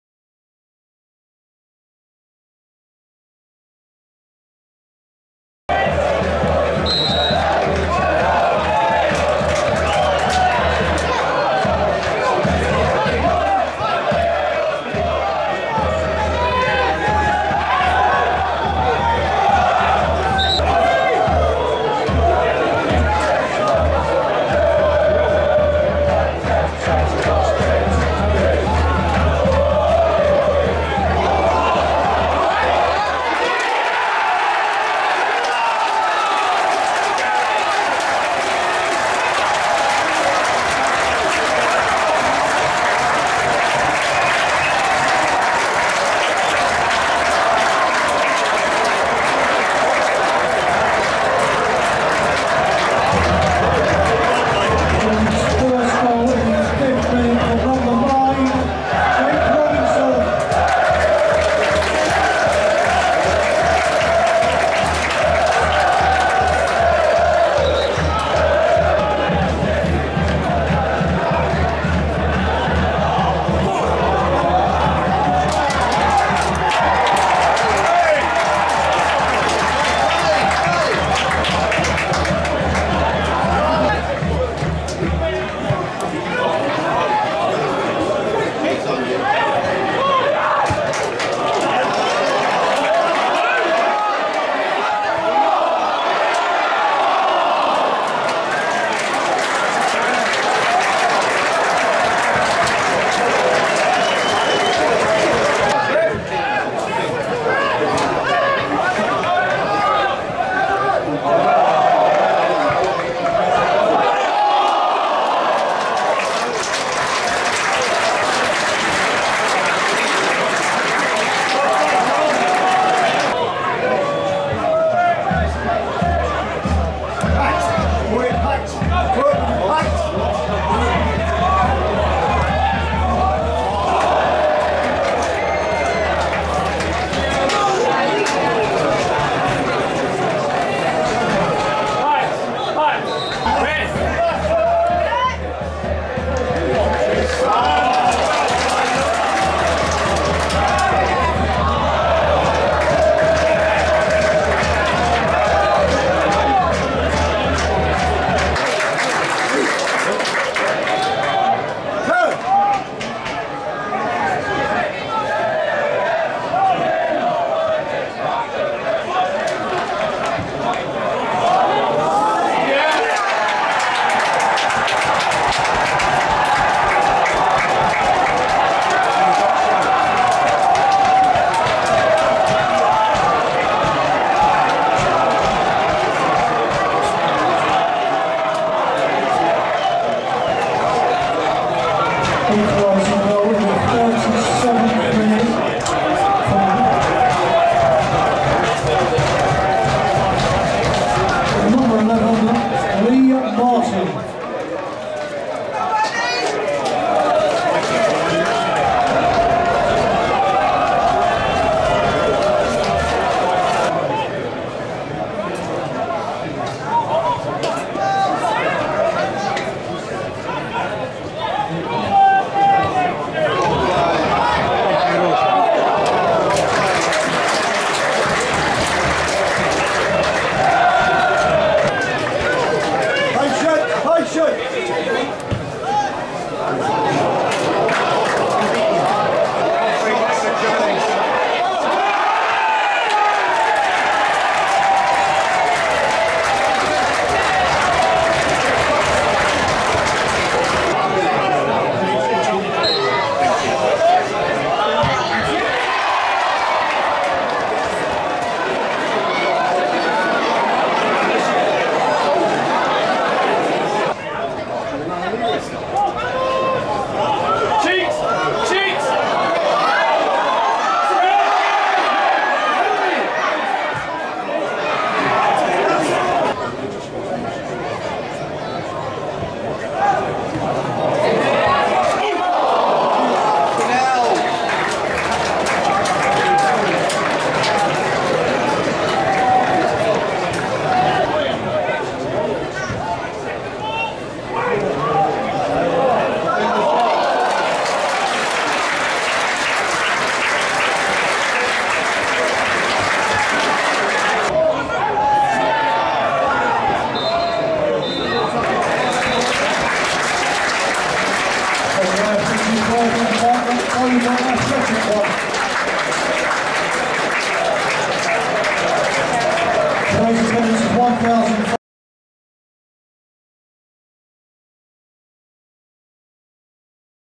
NATIONAL LEAGUE OPENER | Worthing 1-1 Dover | Highlights
Highlights of the National League South fixture between Worthing and Dover Athletic played at the Crucial Environmental Stadium on Saturday 6th August, 2022.